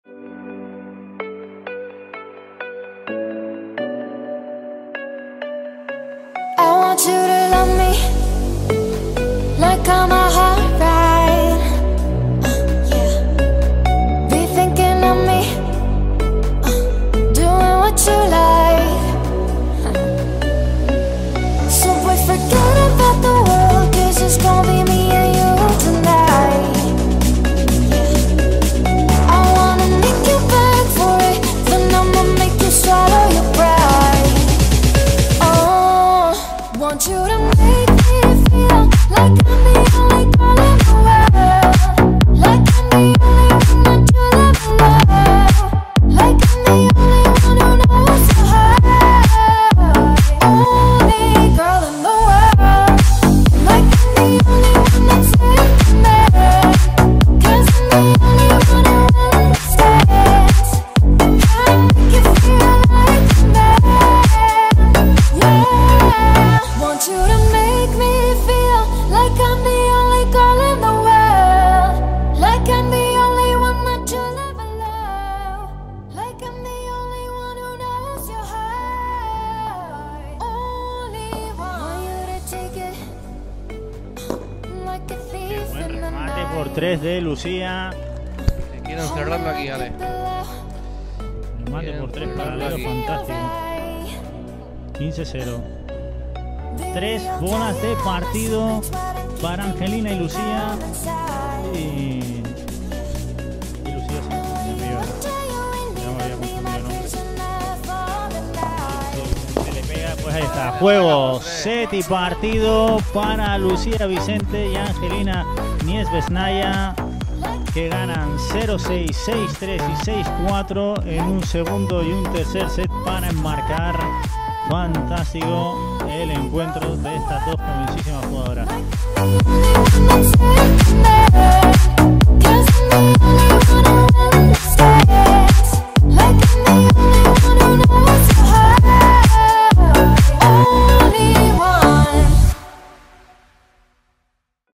Lucia Vicente & Angelina Neizvestna FIP STAR GRAN CANARIAS Agosto 2023
Lucia Vicente y Angelina Neizvestna se meten en cuadro del
FIP STAR GRAN CANARIAS
Agosto 2023